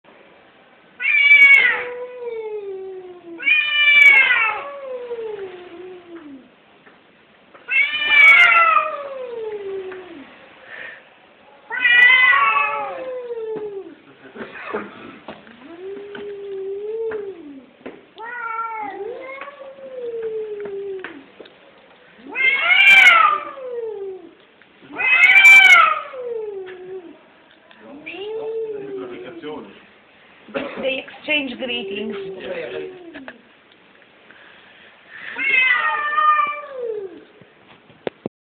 MEOWWWWWWWWWWWWWWWWWW
Two cats are starting to fight in Vernazza XD enjoy!!